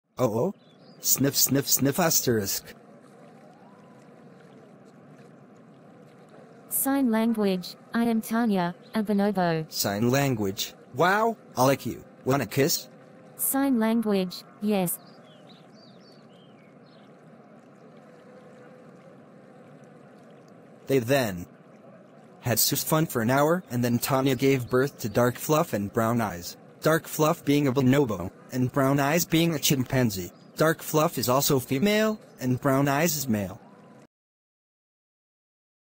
Tony The chimp meets Tania the bonobo!
They become ape parents!